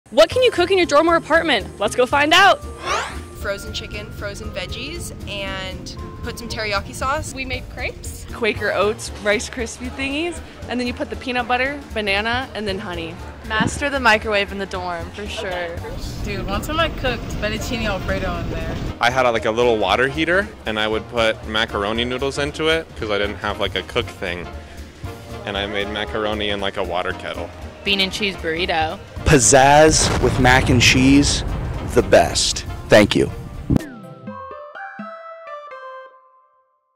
What can you cook in your dorm or apartment? | ASKGCU Grand Canyon University
The crew answers the question, "What can you cook in your dorm or apartment?" If you have a question to ask GCU, use the hashtag ASKGCU.

Approved by the Assistant Vice President of GCU Marketing on June 28, 2023.